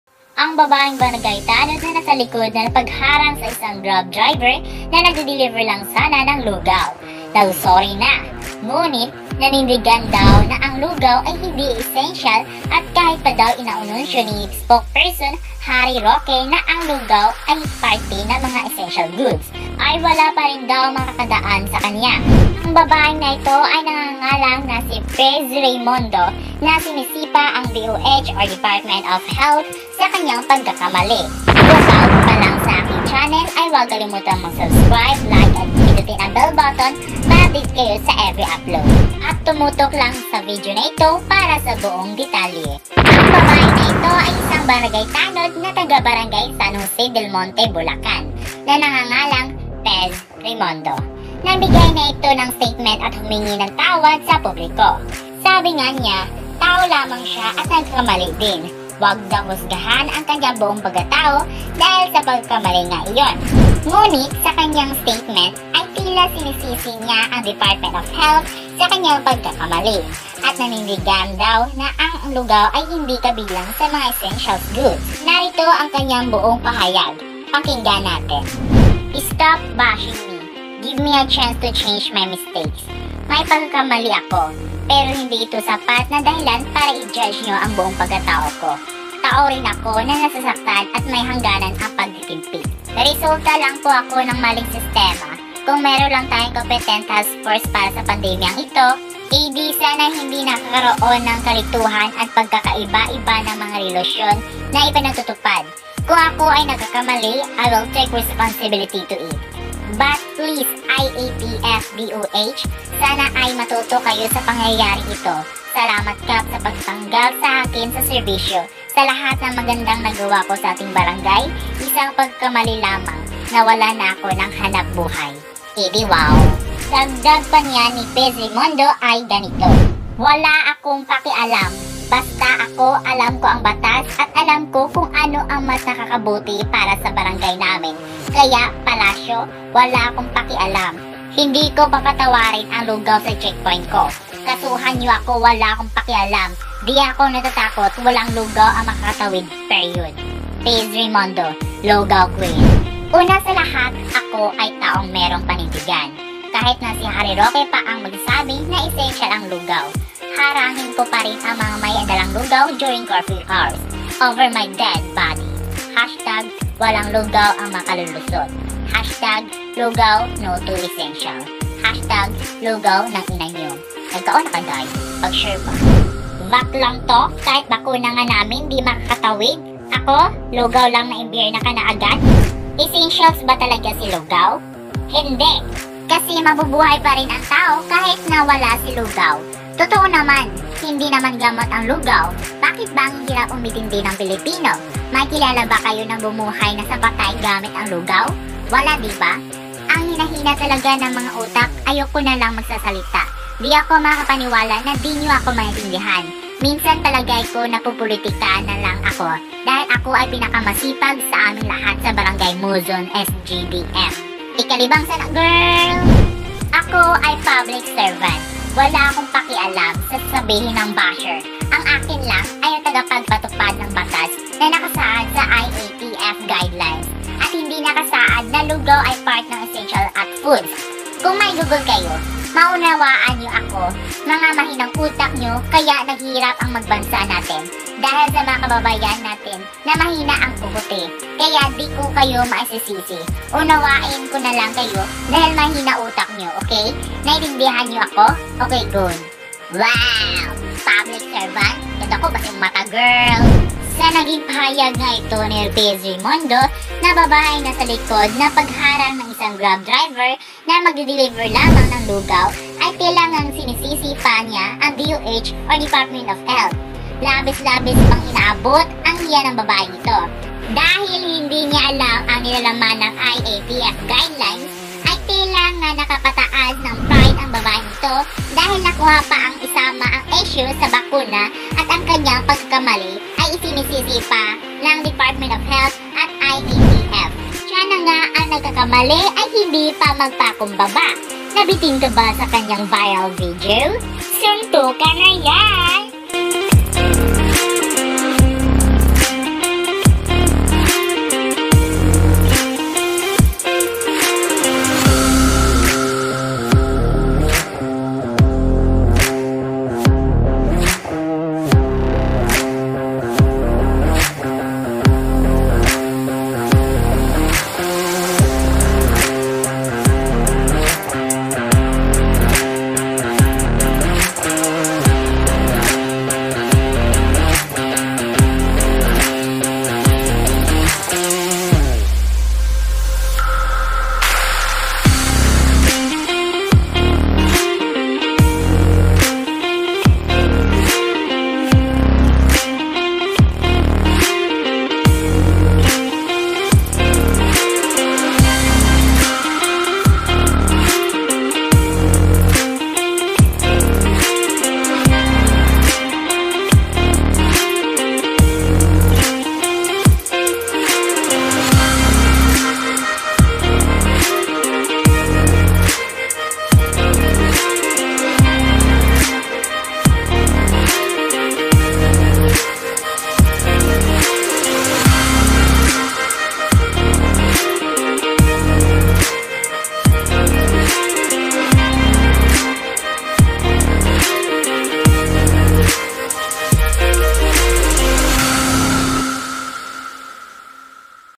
Babaeng nasa likod ng Lugaw Essential. NAGSASALITA NA!
Nagsalita na ang babae na nagviral tungkol sa pag-aabuso ng Isang grab driver na nagdedeliver ng Lugaw.
Panoorin natin to.